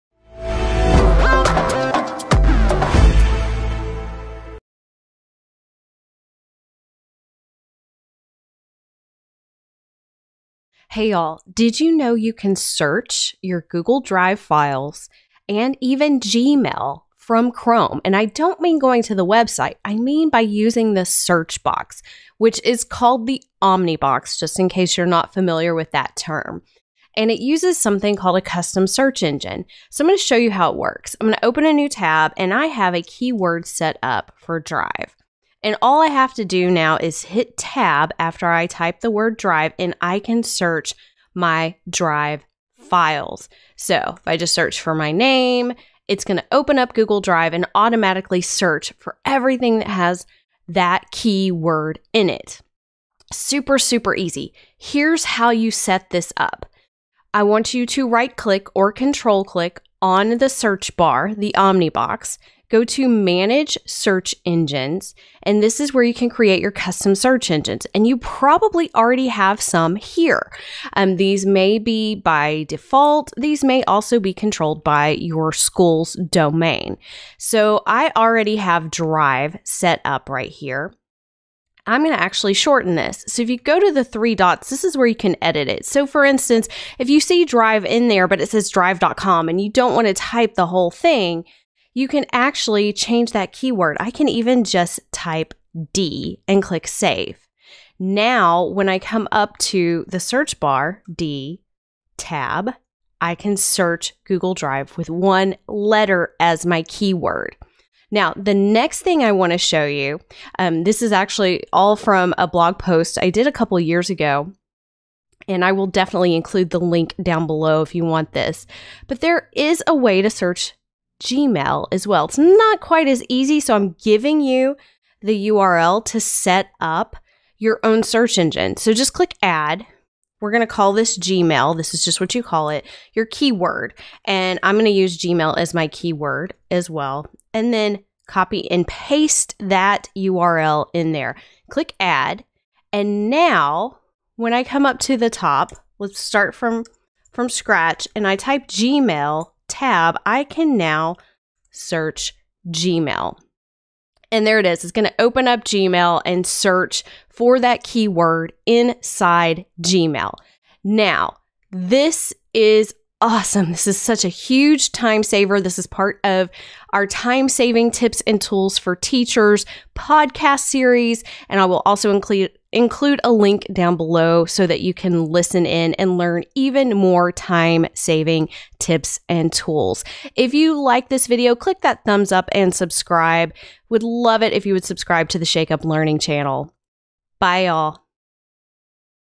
Search Google Drive and Gmail from Chrome!
Did you know you can search your Google Drive from the Chrome address bar?

That’s right! With this one little trick, you can search right from the search box (aka the Omnibox) in Google Chrome.